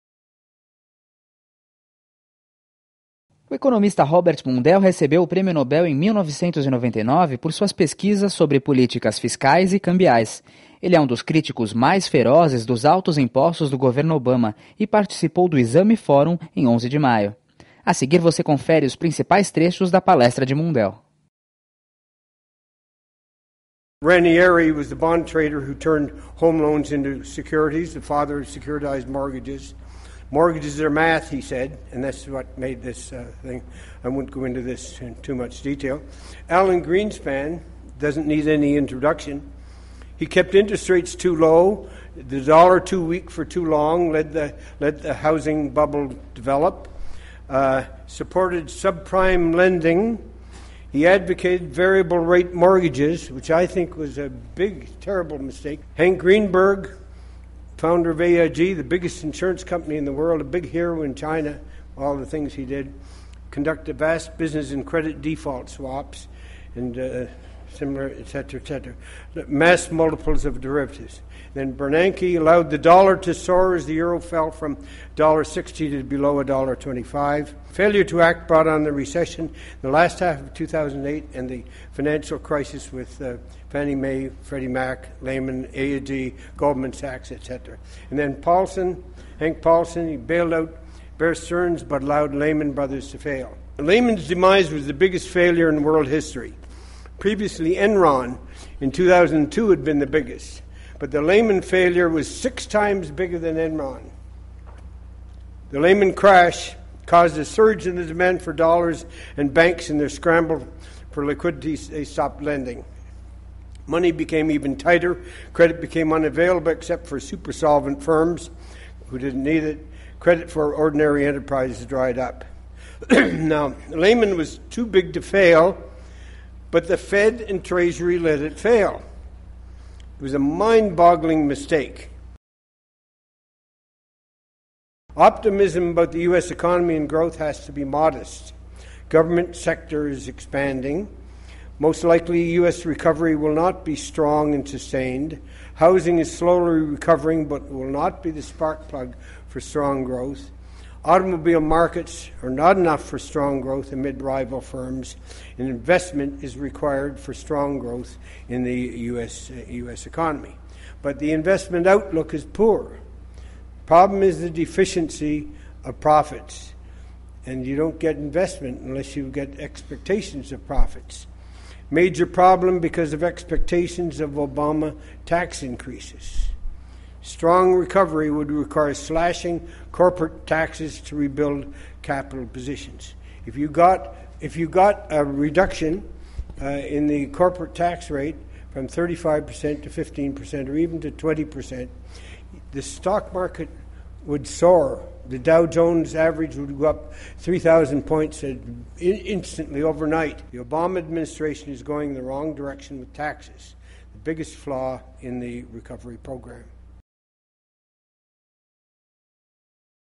Robert Mundell no EXAME FÓRUM 2009 (Parte 1)
O evento da revista EXAME trouxe o americano Robert Mundell (Prêmio Nobel de Economia em 1999), um dos críticos mais ferozes dos altos impostos do governo Obama.

EXAME FÓRUM 2009, realizado pela revista EXAME (Editora Abril), ocorreu dia 11 de Maio de 2009, no Hotel Unique, em São Paulo. Com a presença exclusiva de três Prêmios Nobel, Delfim Neto e José Serra, eles debateram a crise global e as alternativas para a reconstrução da economia nacional e mundial.